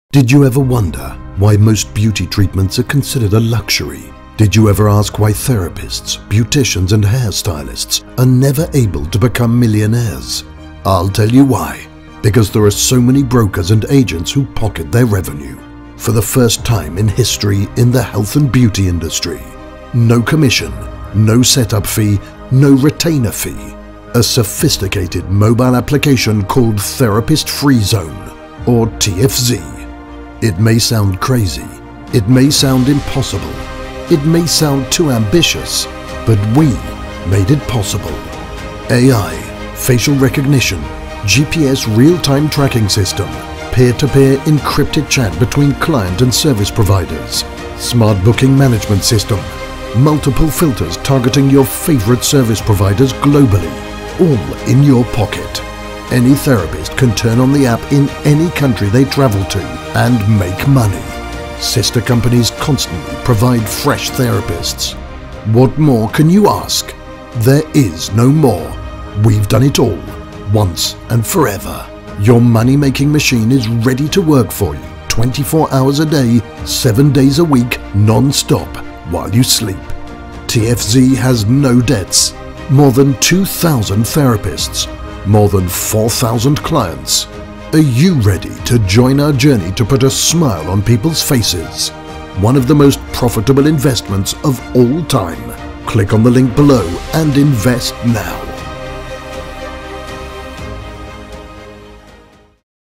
TFZ or Therapists Free Zone, Triler 2
TFZ or Therapist Free Zone is a sophisticated, high tech global Health, Holistic & Beauty Mobile Application.

We provide FREE Marketing for shops, mobile freelancers, beauticians, and technicians around the world.

TFZ is the first-ever secure platform globally that allows clients to freely find a therapist, book and pay, then track them in real-time without any broker or a massive commission.
Thanks to the in-house developer and his team TFZ will be a fully automated system using AI technology.

Youtube video for investment

Now if you are interested fill the following form and I will contact you for the next step.
(this is our sister company which support and supply us at all time)